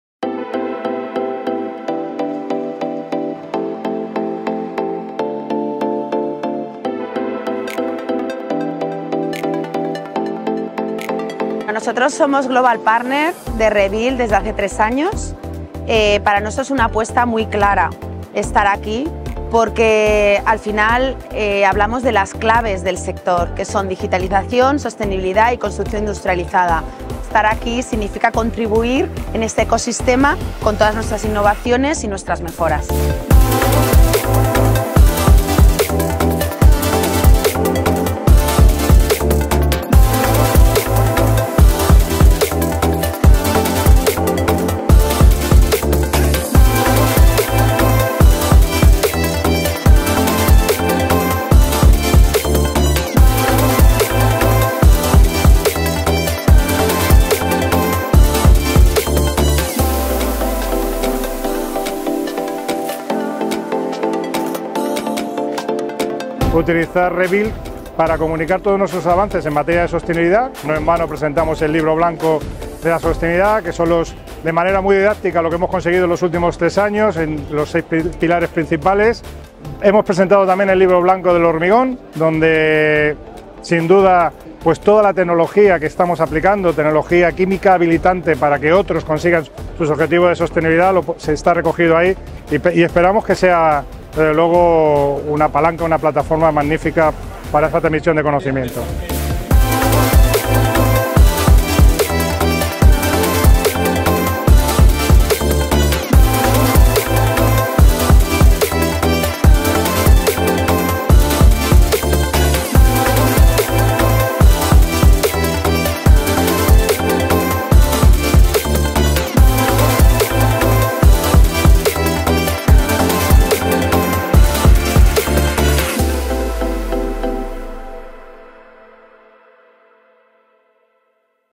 Rebuild 2023: Sika Global Partner
Sika acudió a Rebuild 2023 como Global Partner por tercer edición consecutiva. Con el objetivo de dar a conocer el hormigón sostenible y aportando todo su conocimiento en la materia con entrevistas, mesas redondas y ponencias.